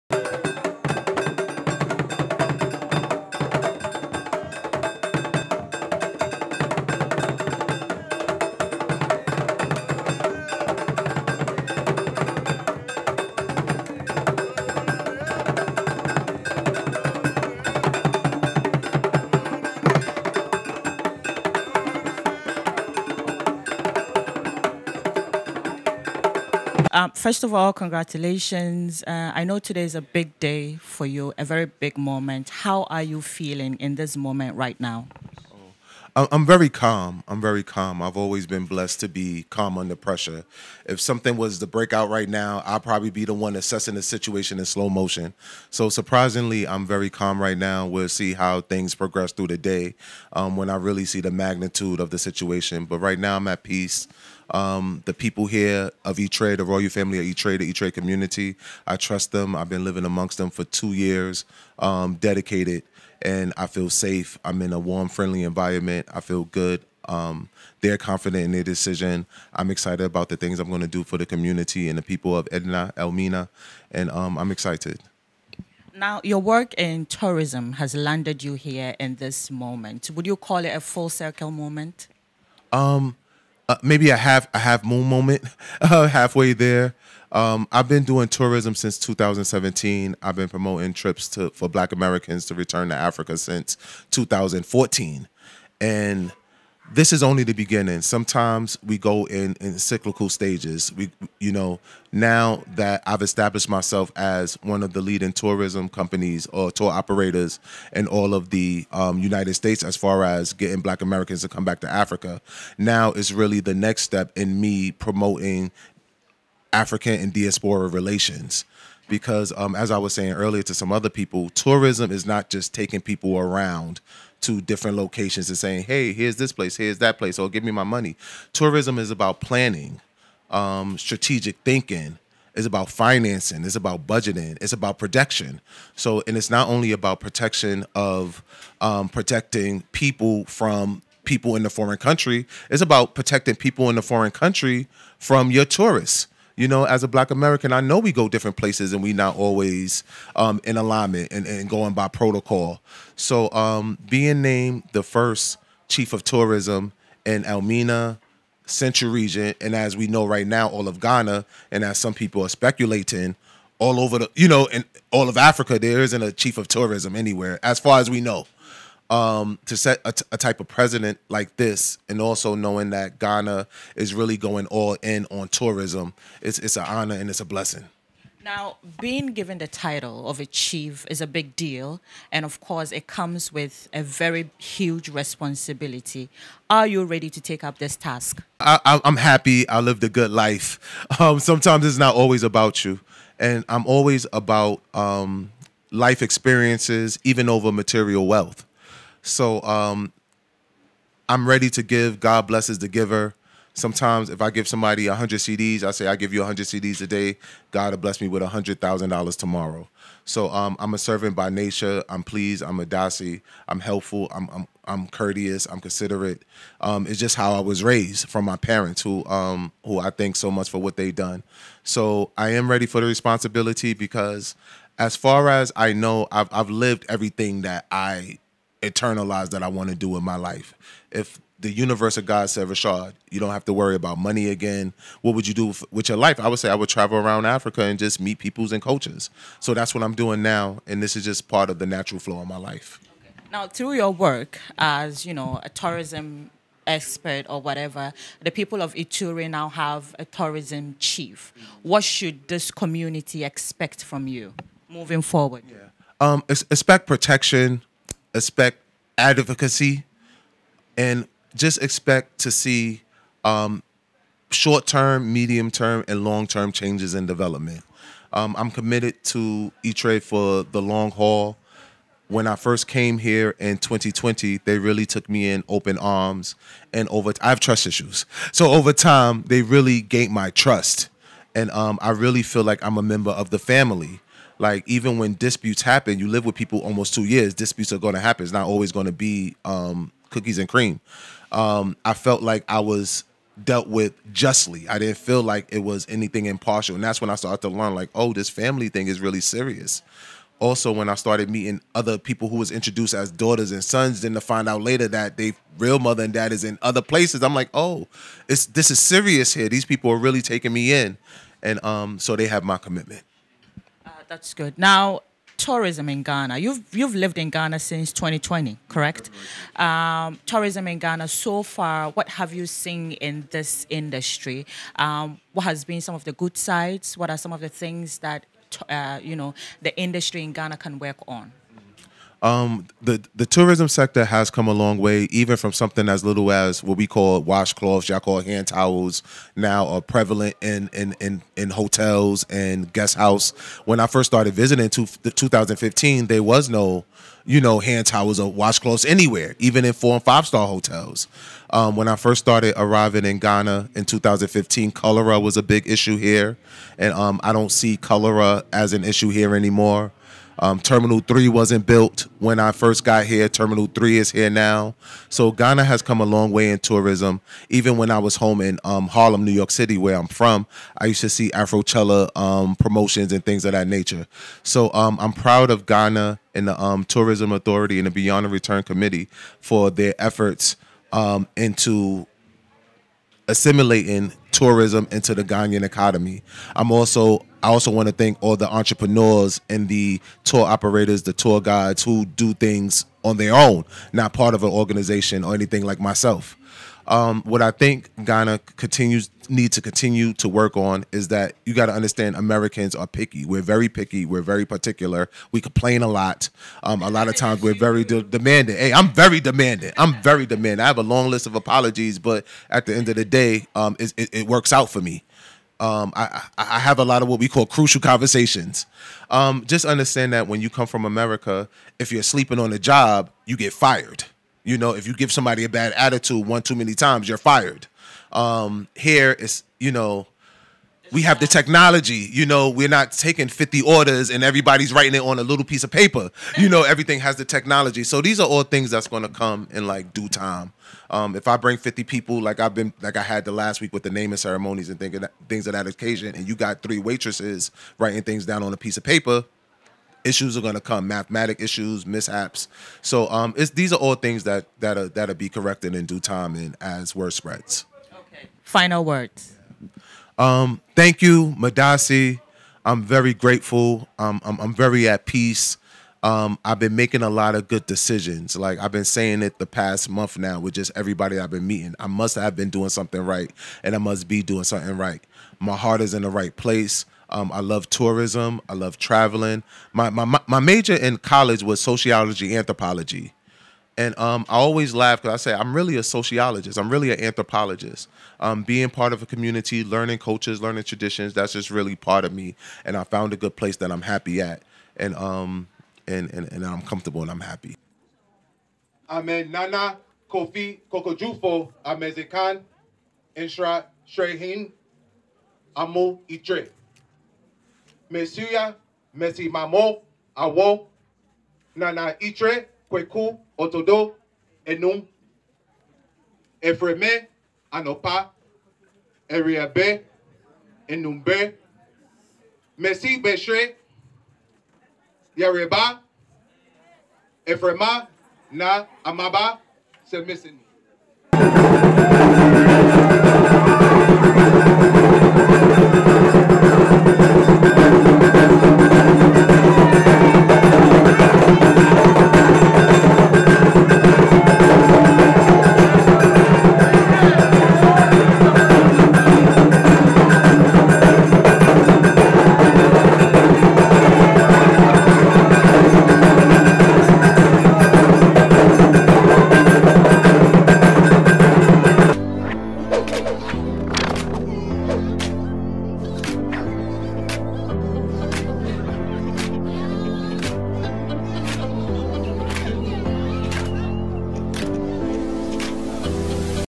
Rashad McCrorey enstooled Nsrehwehen (Tourism Chief) of Iture/Elmina
Ameyaw TV brings you up to date with news, exclusive interviews, live shows, events, and lifestyle shows among others